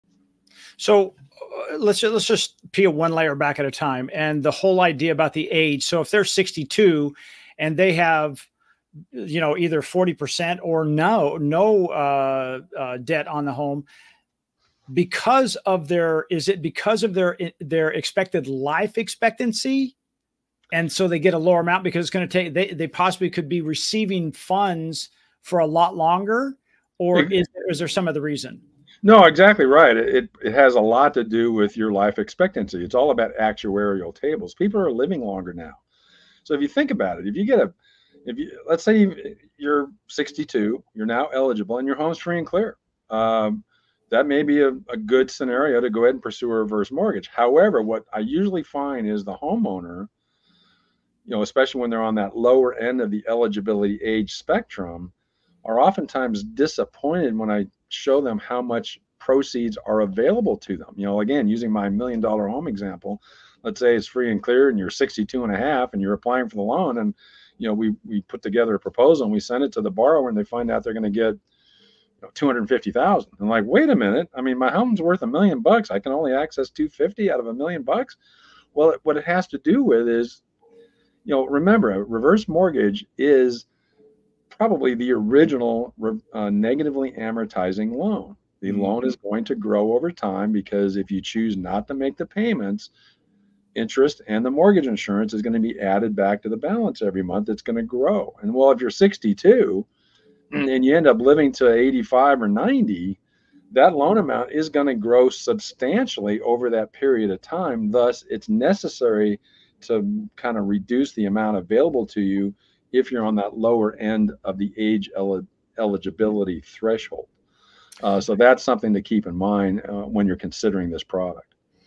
Understanding Reverse Mortgage Proceeds: Why Homeowners Might Be Disappointed
Considering a reverse mortgage? Here's a crucial insight you need! Many homeowners, especially those at the lower end of the eligibility age spectrum, often feel disappointed when they see how much proceeds are available.

For example, if you have a million-dollar home, are 62 and a half, and applying for a reverse mortgage loan, you might find out that you can only access $250,000. This is because reverse mortgages grow over time due to interest and mortgage insurance being added back to the balance each month.

If you live to 85 or 90, that loan amount can grow substantially. Thus, reducing the amount available to younger applicants helps prevent future financial strain.

Understand this before making your decision!